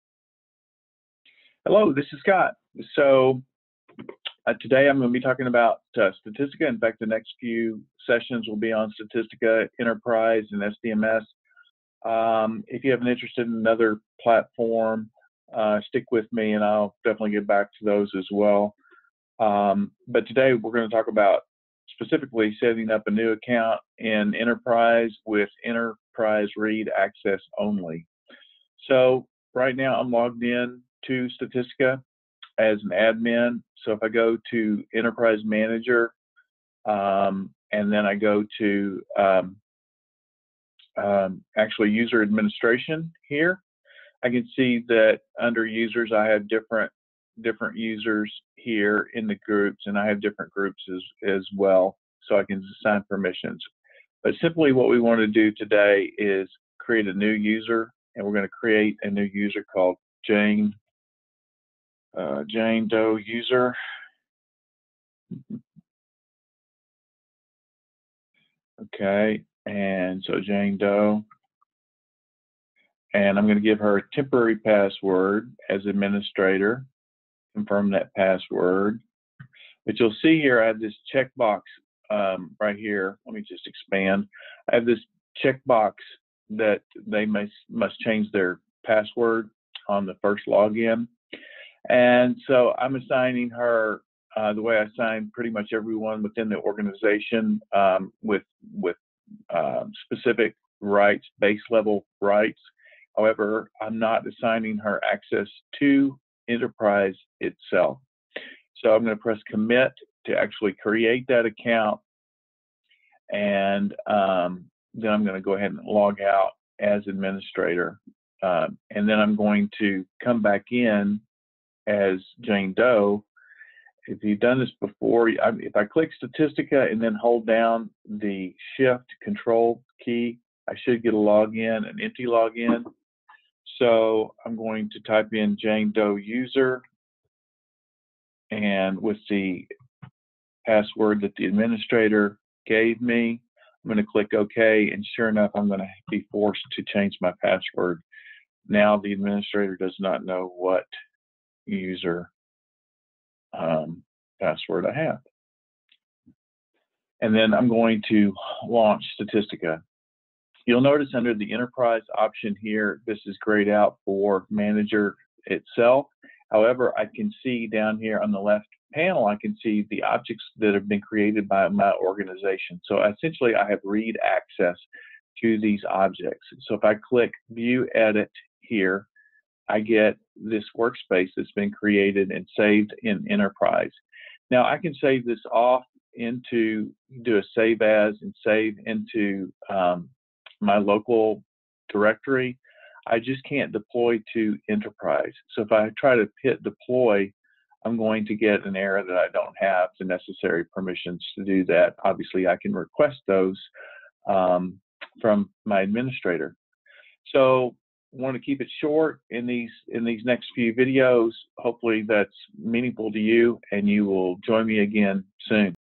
S51 JaneDoeUser – Setting Up an New Account in Statistica Enterprise with Enterprise Read Access
Whether you are an Analytics Manager, Data Scientist, Analyst or Engineer you will love Statistica.  This video covers how to set up a new account in Statistica with Enterprise read only access.